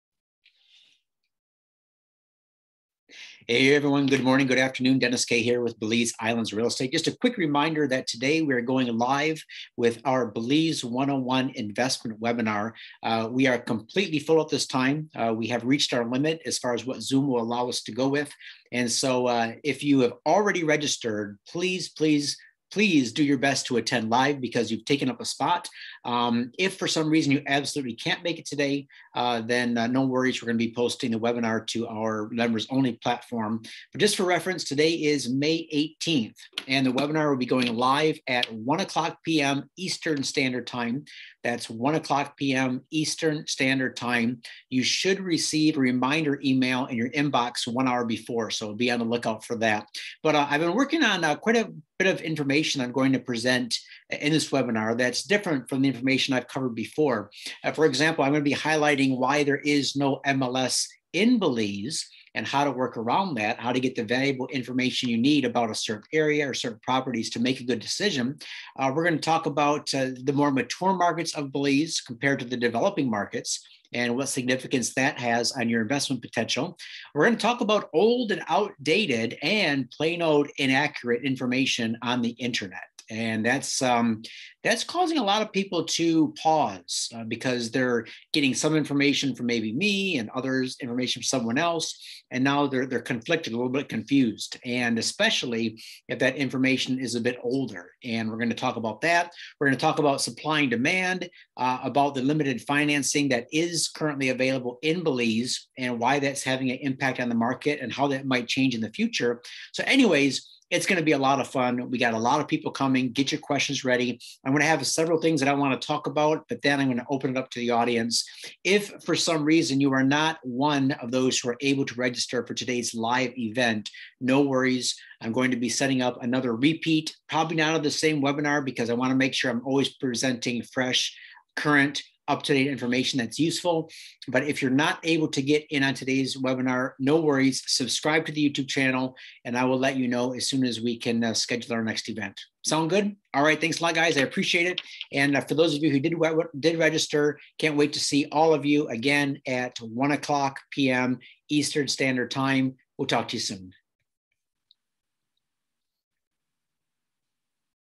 Belize 101 Live webinar event is today (thinking of moving to Belize or investing offshore)
Our Belize 101 live webinar event is today. Congrats to all who were able to register.

For those unable to get on todays live event we will be hosting another one in the next 1-2 weeks.

Thanks and see you soon!

Dennis Kay - Helping people move to and invest in Belize real estate